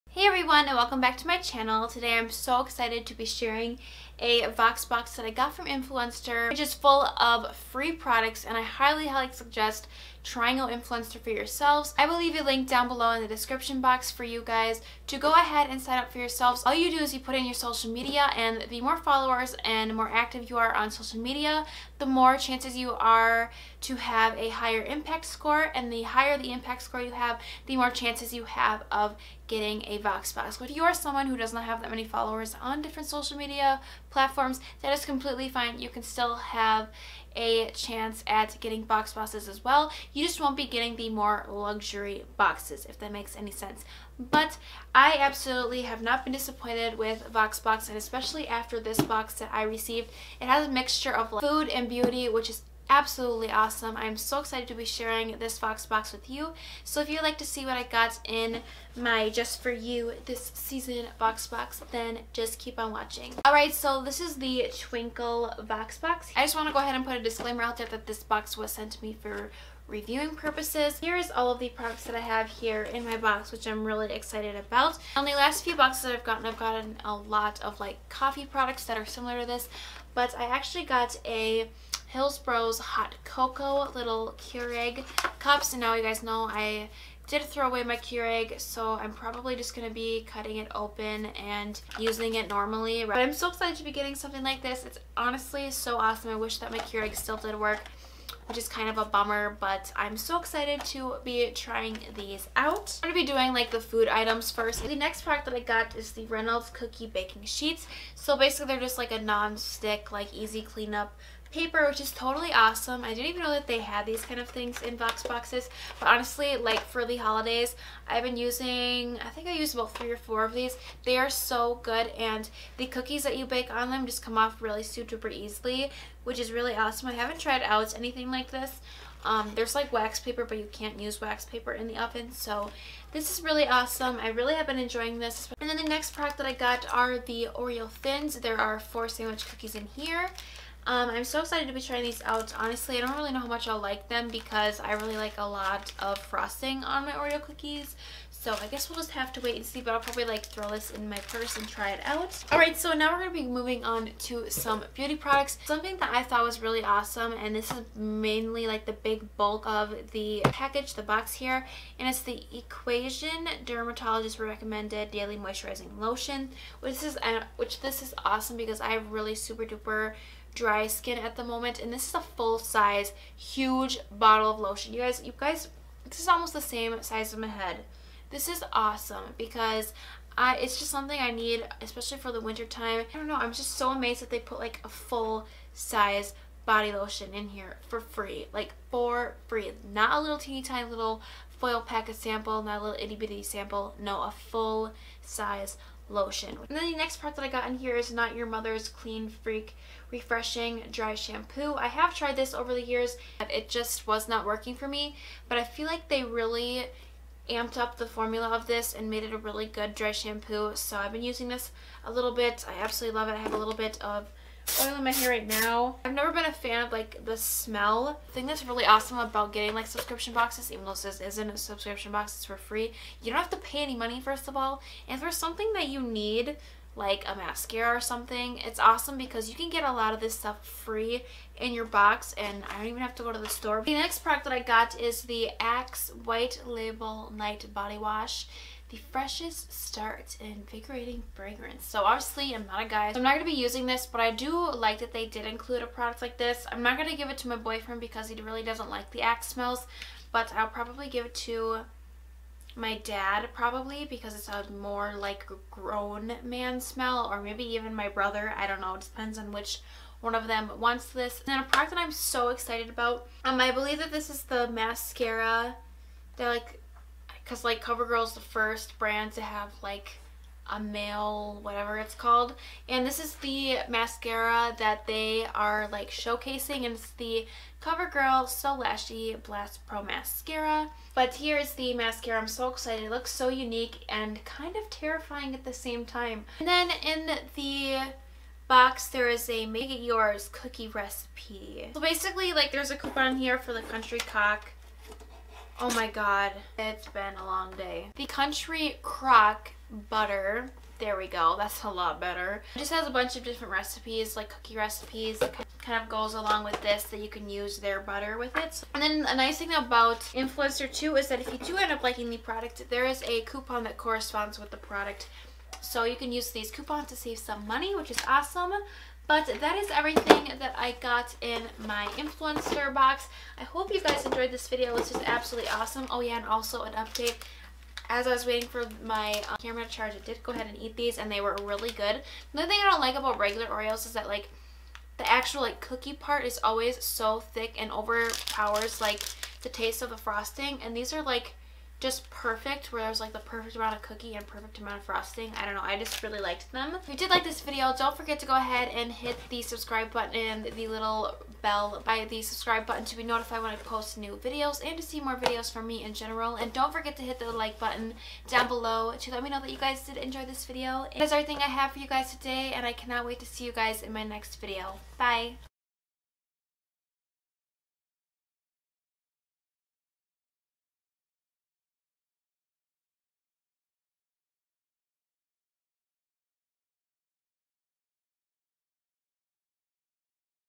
Twinkle Influenster Unboxing | Beauty With Krista
This box was so  good!  The best part is that it was filled with a varitey of products that were for free!  The link to sign up is down below:

Go follow Dodo Lashes on Instagram: @dodolashes

If you would like to send me something, here is where you can do that: